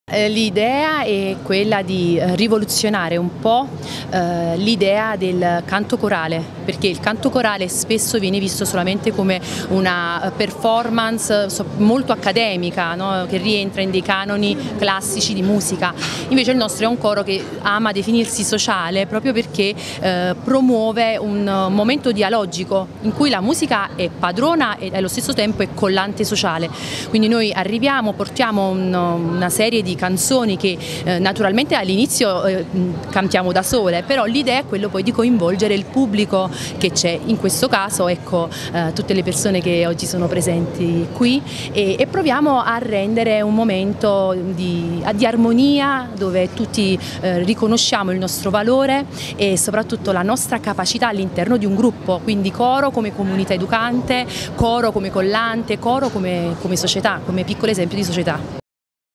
MUSICA AL CARCERE, DIRETTRICE CORO MAZZONE: “MOMENTO DI AGGREGAZIONE COLLETTIVA”
Si è tenuto presso la casa circondariale di Benevento uno spettacolo musicale per promuovere momenti di condivisione musicale creando scambi dialogici in cui la musica diventa innanzitutto collante sociale più che momento di pura esibizione.
La direttrice del coro “Incanto”nato nel 2013, Luciana Mazzone, ha dichiarato: “una giornata scandita dalla musica, da occhi che incrociano altri occhi, da ritmo e risate contagiose. Uno spettacolo musicale con un repertorio multietnico e classico.”

Ascoltiamo i dettagli delle sue dichiarazioni ai microfoni di TV7.